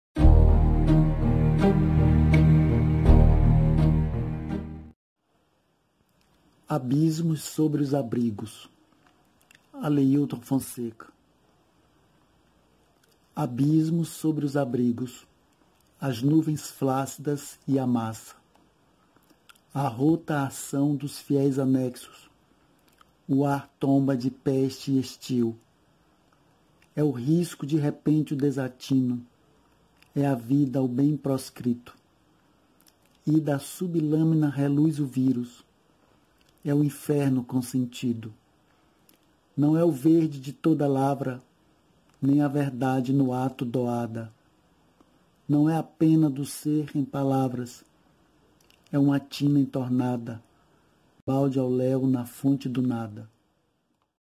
O Recanto do Poeta com Aleilton Fonseca - Abismos sobre os abrigos
Em mais uma edição da série de vídeos do Recanto do Poeta com Aleilton Fonseca, temos desta vez o poema "Abismos sobre os abrigos" de autoria do nosso querido escritor.